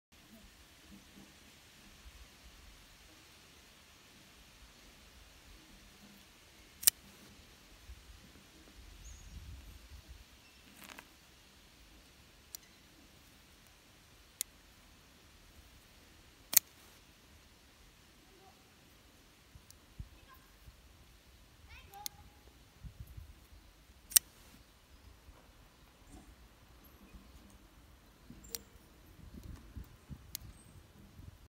HX Outdoors ZD022 blue
Action of the Rike made HX-Outdoors ZD022 ( mini Armorhide ). Titanium framelock s35vn steel blade